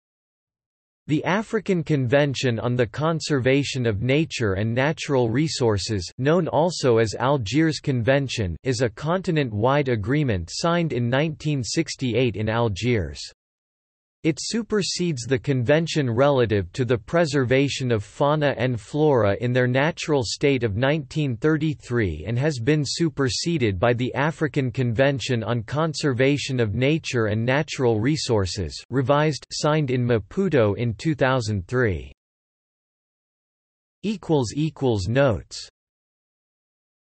African Convention on the Conservation of Nature and Natural Resources | Wikipedia audio article
- Socrates

SUMMARY
The African Convention on the Conservation of Nature and Natural Resources (known also as Algiers Convention) is a continent-wide agreement signed in 1968 in Algiers. It supersedes the Convention Relative to the Preservation of Fauna and Flora in their Natural State of 1933 and has been superseded by the African Convention on Conservation of Nature and Natural Resources (revised) signed in Maputo in 2003.